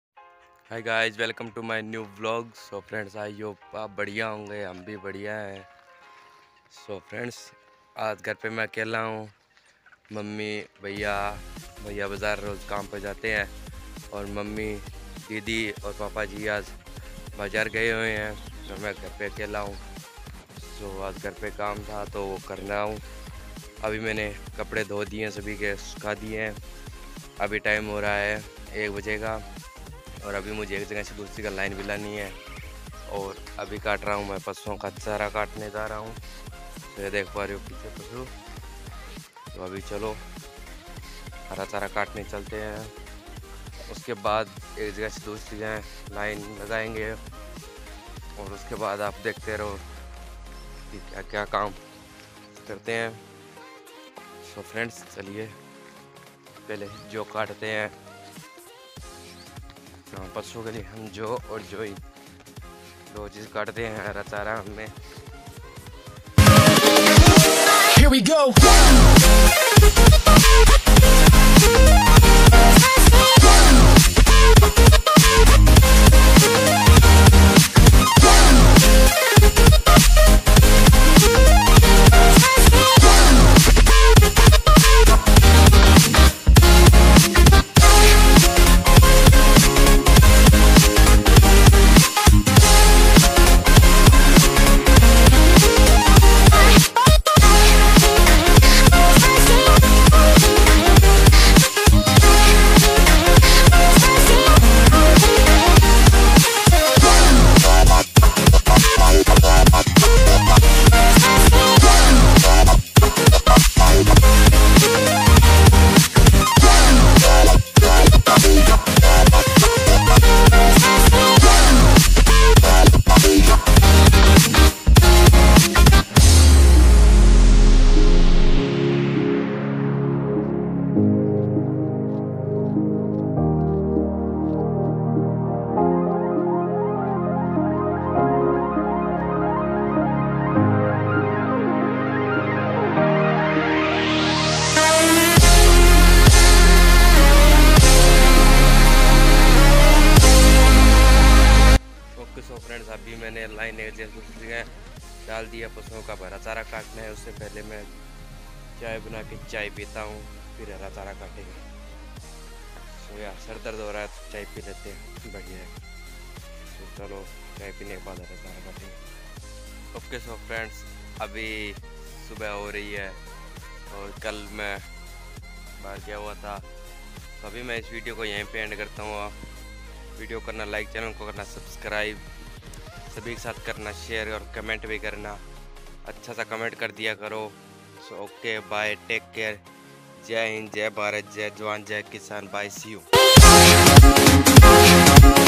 Gar par akala ka kam🙂
aj gar ka kam  kia
gar par akala rahana ka bad Sara kam kia

Har chara kata